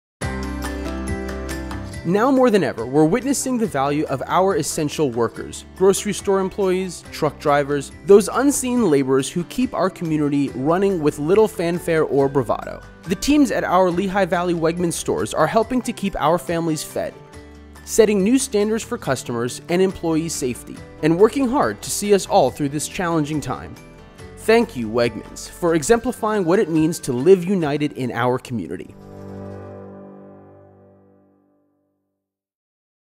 Campaign Chairs' Award - Wegmans | UWGLV
Thanks to our partnership with Wegmans and their checkout campaign we welcomed almost 1,000 new donors!

That’s one reason why we’re proud to recognize Wegmans with our annual Campaign Chairs’ Award. Our sincere thanks for the many years of partnership and for everything your team is doing to keep our Greater Lehigh Valley safe and healthy.